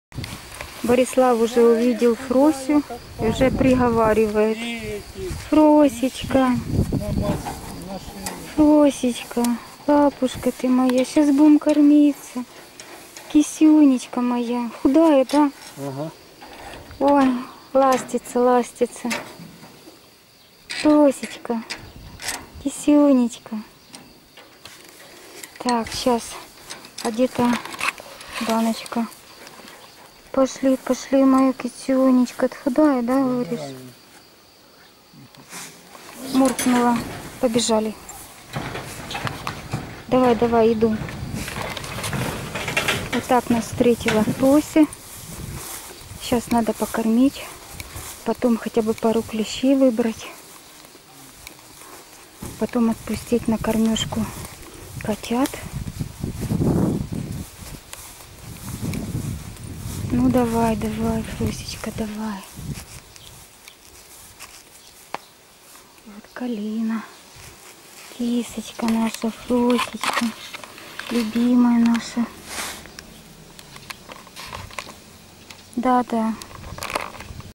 Встреча с худой Фросей
Приехали.Во Дворе никого из людей не было.Нас встретила у ворот Фрося-Ефросинья.Худая,как сразу отметил Борислав.Не могу ничего с собой поделать от мысли печальной при виде голодных животных ,и особенно тех,у кого есть Хозяева. А ведь Фросе ещё надо выкармливать ТРОИХ Котят !Откуда у НЕЁ выработается молоко,если Кошка сама голодная?.